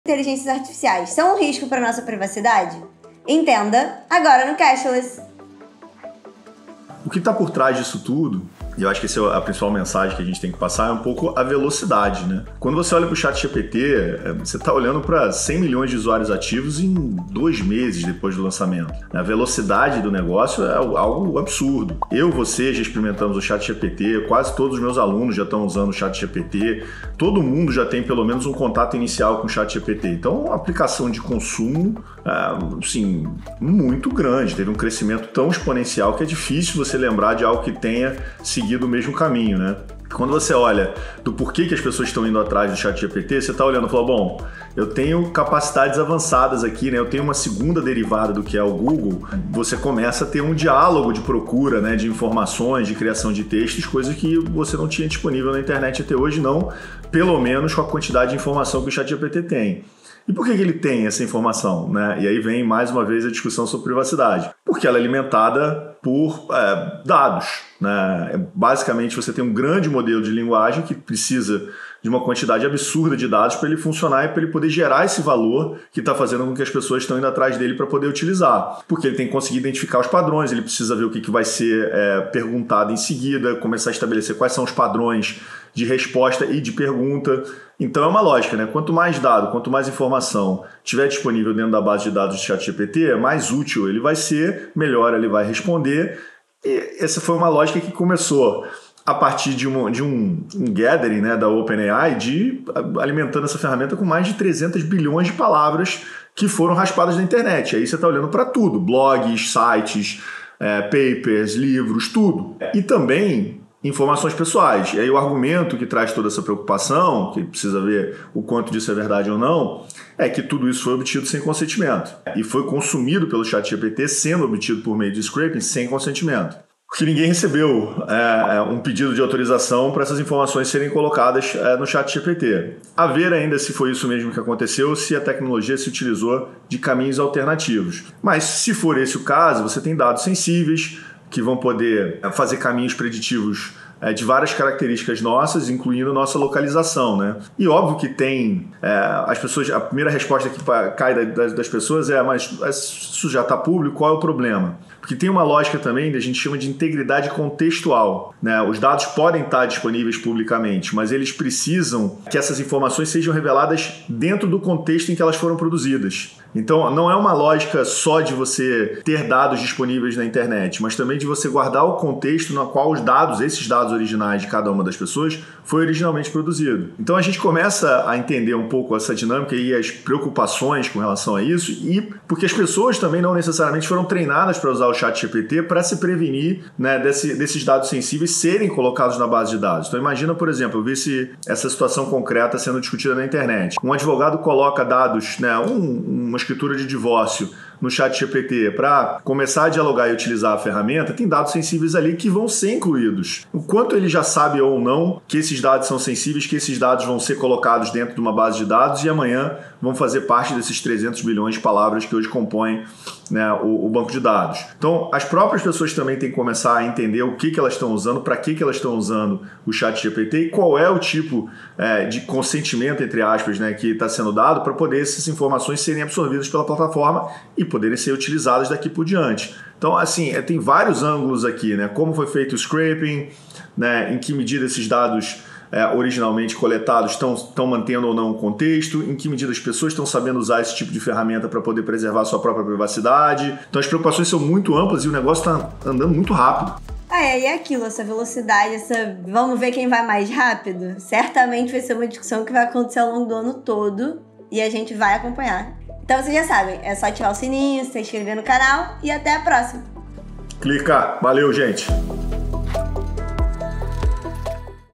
ChatGPT: estamos prontos para essa inteligência artificial?
ChatGPT banido da Itália por violar regras de privacidade e uso dos dados. Foi o primeiro país do Ocidente a proibir o uso da ferramenta e levanta debates.

Entenda como isso pode afetar o mundo.

-  O ChatGPT vai mudar as relações de trabalho? Tudo sobre a nova inteligência artificial do momento